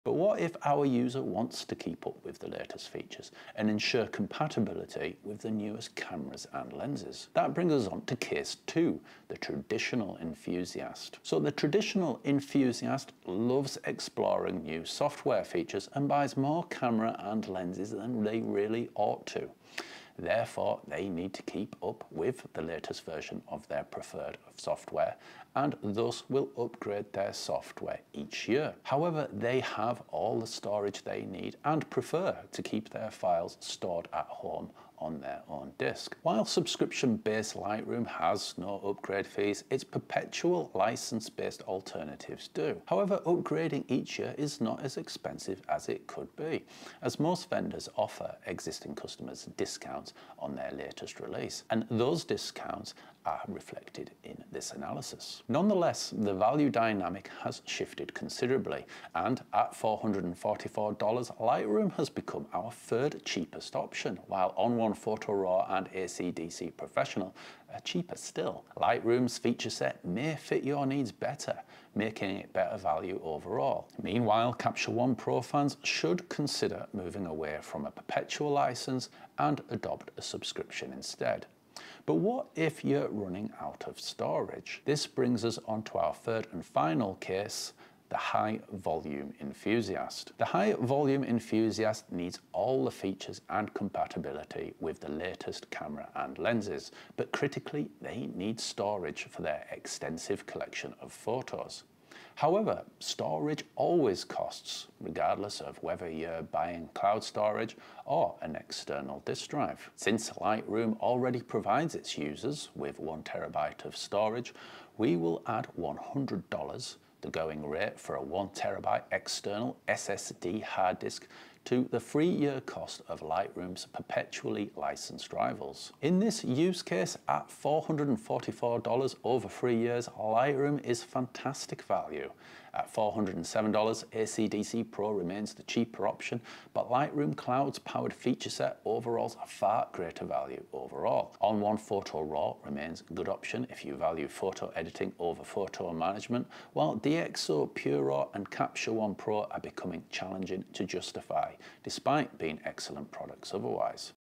Best Photo Editing Software: Lightroom vs. Alternatives
Software upgrade analysis reveals surprising values! Lightroom's powered features offer far greater value, especially when considering storage and compatibility. Enthusiasts may find ON1 Photo RAW a good option if they value photo editing over photo management.